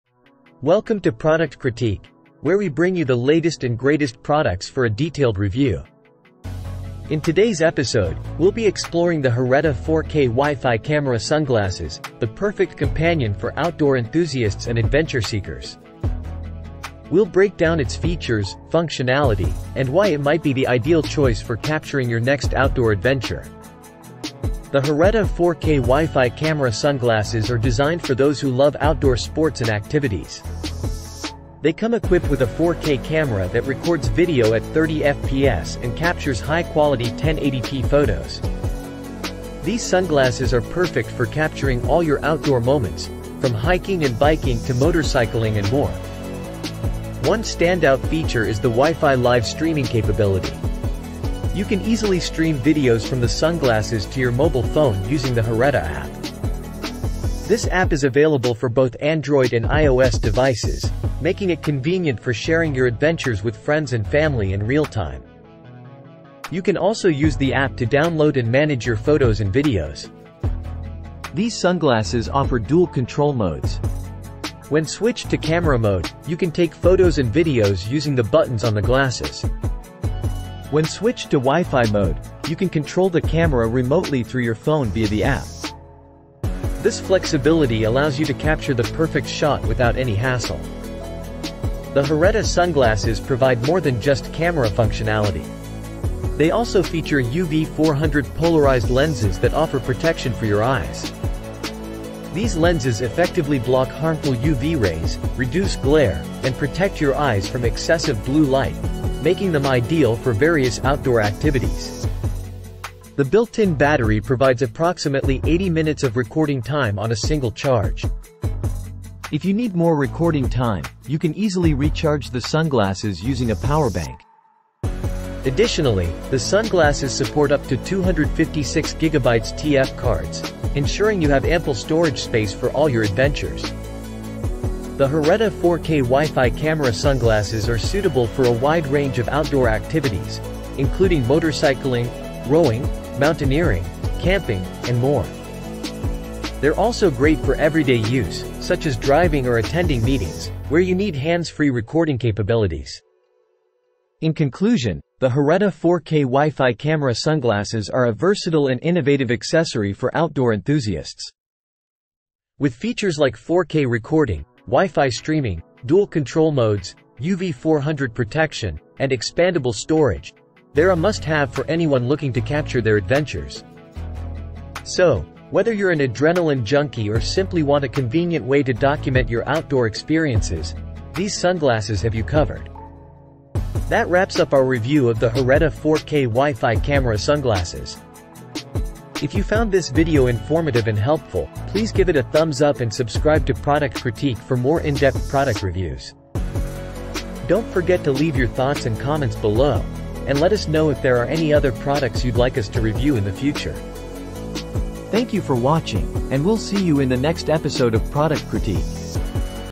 🕶️ Hereta 4K WiFi Camera Sunglasses Sports HD 1080P Video Recording Glasses DVR Eyewear Review 📷
Amazon Product Name : Hereta 4K WiFi Camera Sunglasses Sports HD 1080P Video Recording Glasses DVR Eyewear

Discover the perfect blend of style and technology with the Hereta 4K WiFi Camera Sunglasses. These sports-inspired eyewear not only shield your eyes from the sun but also feature HD 1080P video recording capabilities. With built-in WiFi, you can easily connect and transfer your footage, making it convenient for all your outdoor adventures. Capture life's moments hands-free with Hereta 4K WiFi Camera Sunglasses, the ultimate DVR eyewear. Whether you're on a thrilling outdoor adventure or simply want to record your everyday experiences, these sunglasses are your ideal companion. Experience the future of eyewear technology with Hereta 4K WiFi Camera Sunglasses.